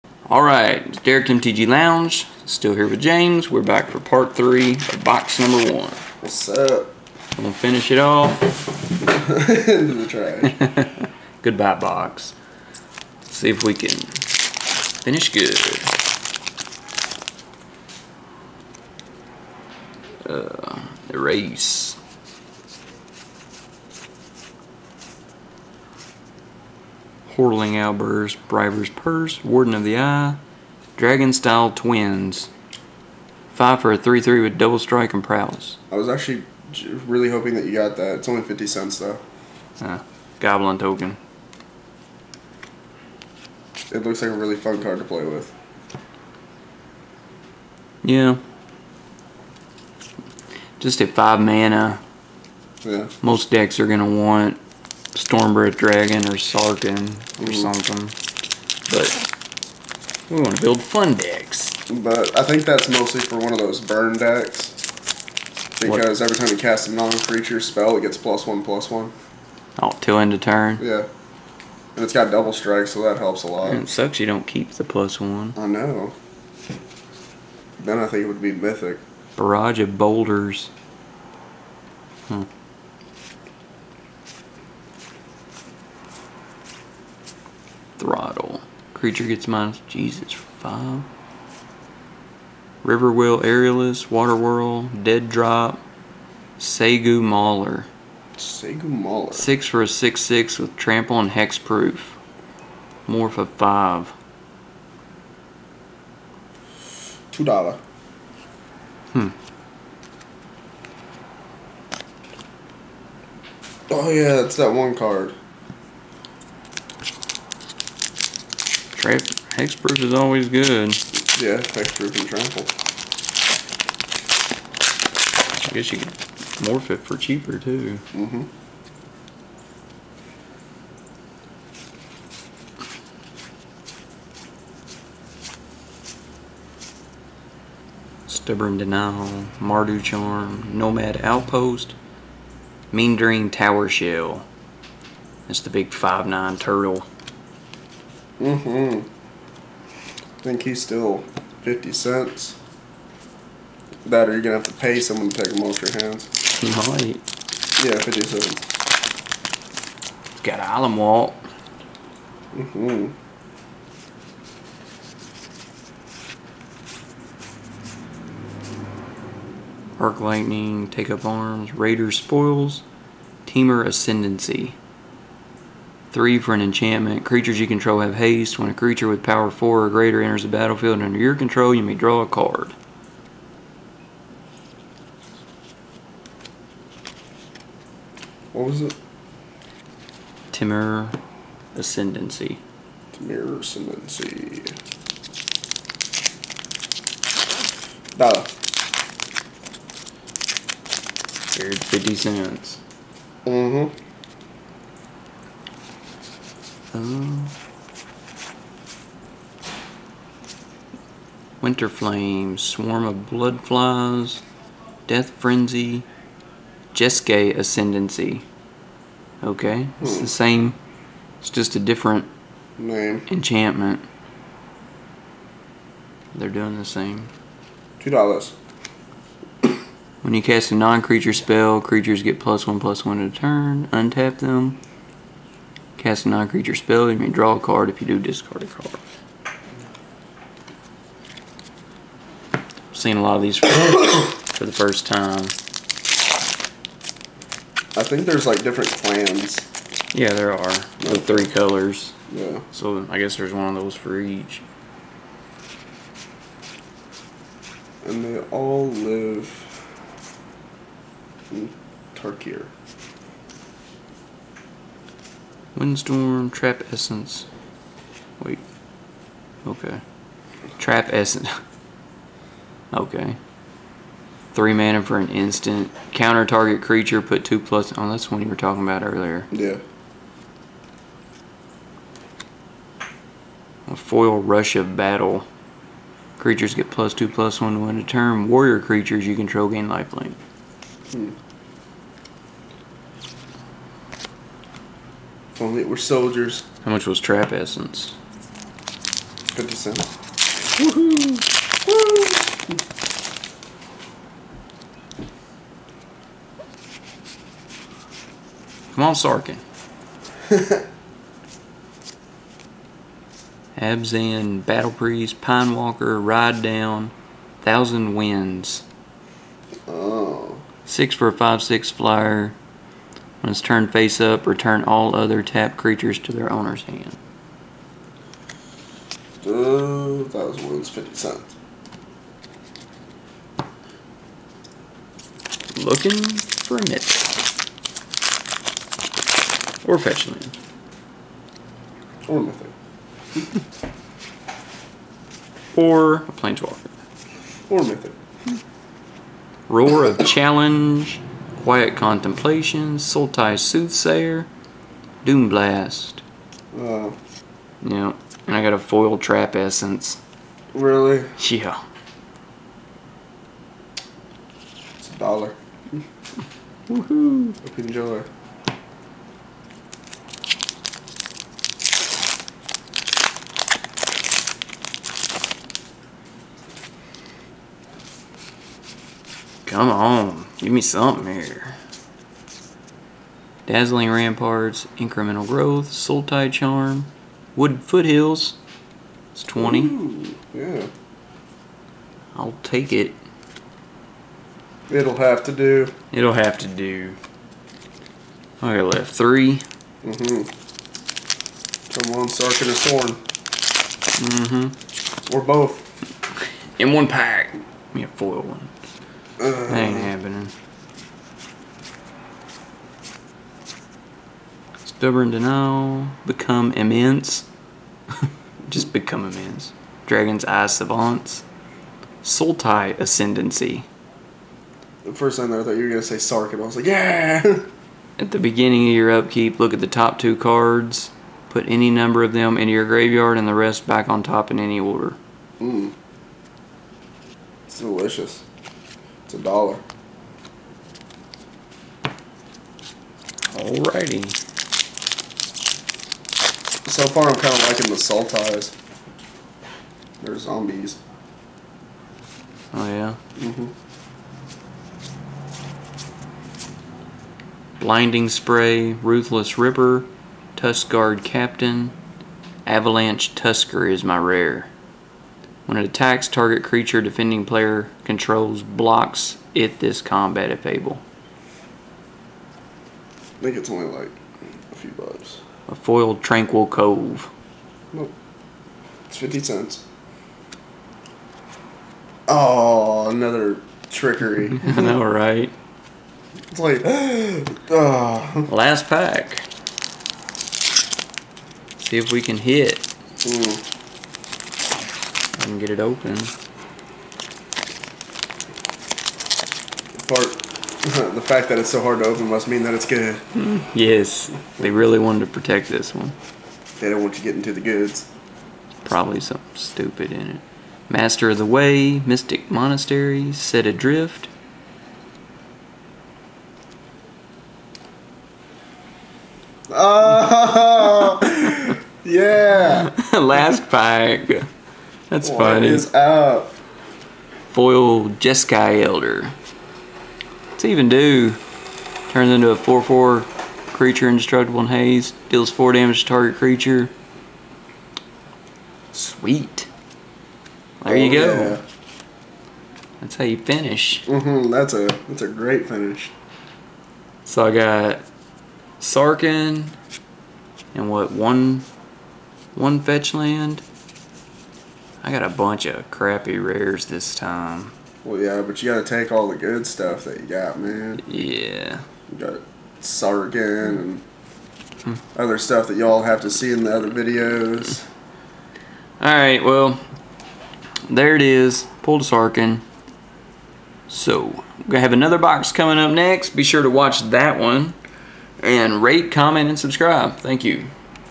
MTG Khans of Tarkir Booster Box Opening #1 (Part 3/3)
The new set is out. Watch and find out what Derek pulls. MNYESS!!